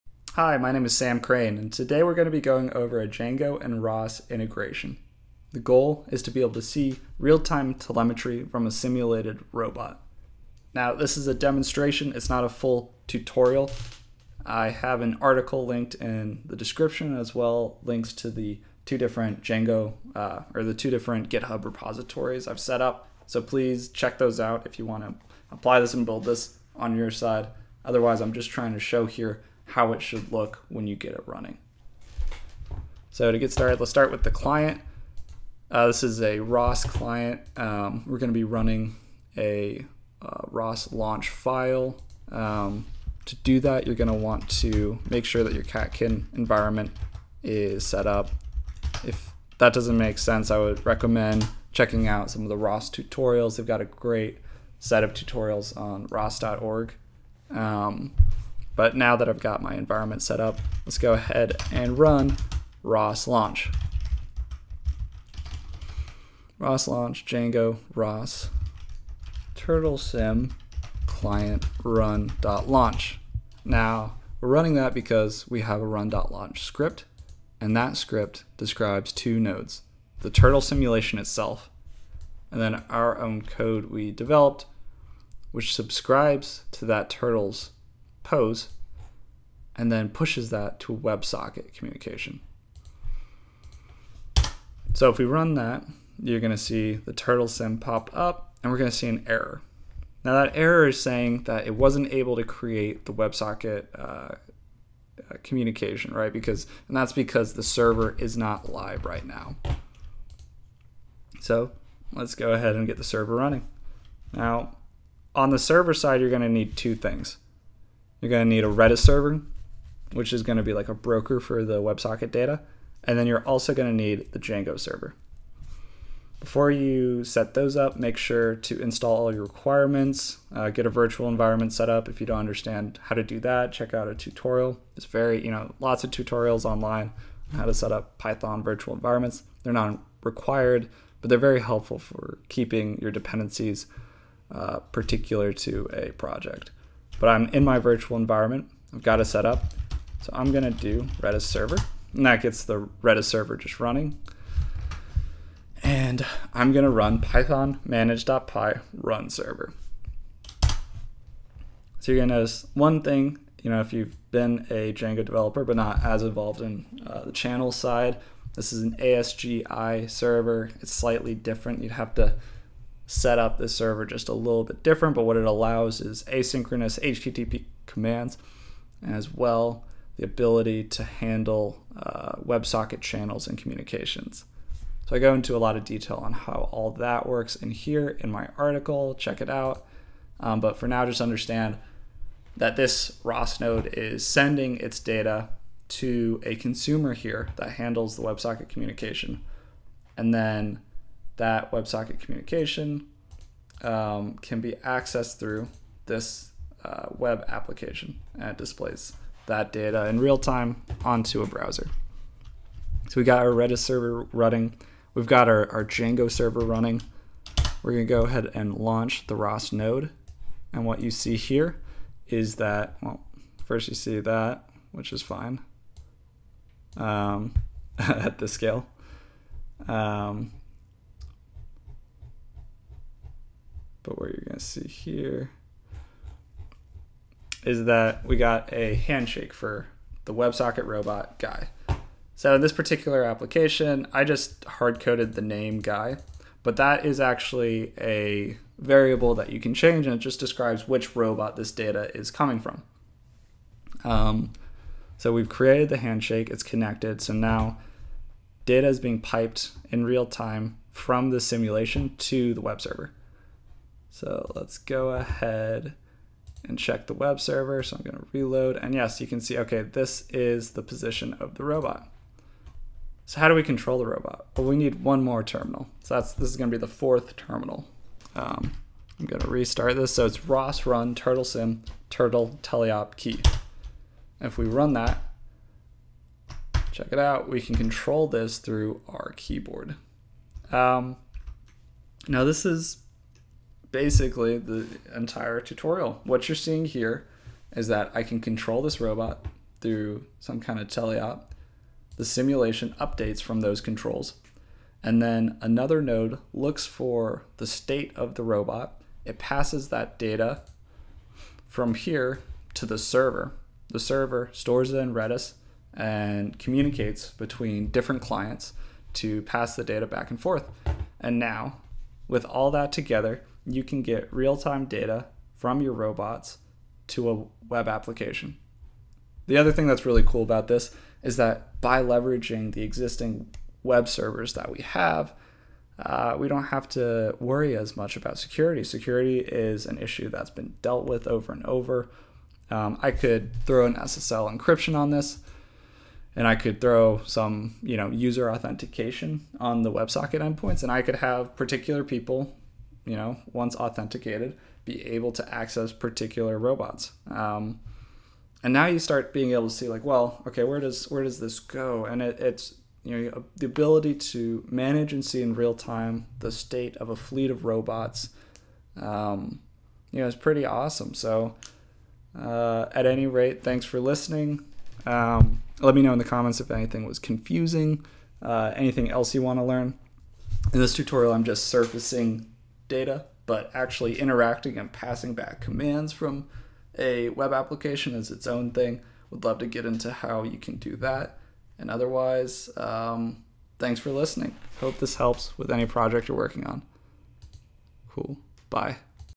Tutorial: Displaying Real-time Robotic Operating System (ROS) Data with Django
Today I will be demonstrating code from one of my articles on how to integrate the Robotic Operating System with Django.